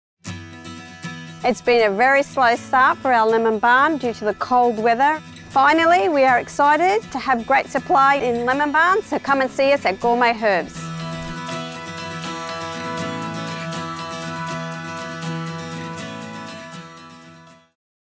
Gourmet Herbs - Lemon Balm
At Gourmet Herbs, we offer a variety of herbs,  including our Lemon Balm Web. Watch this video to find out more about how this herb can compliment gourmet dishes.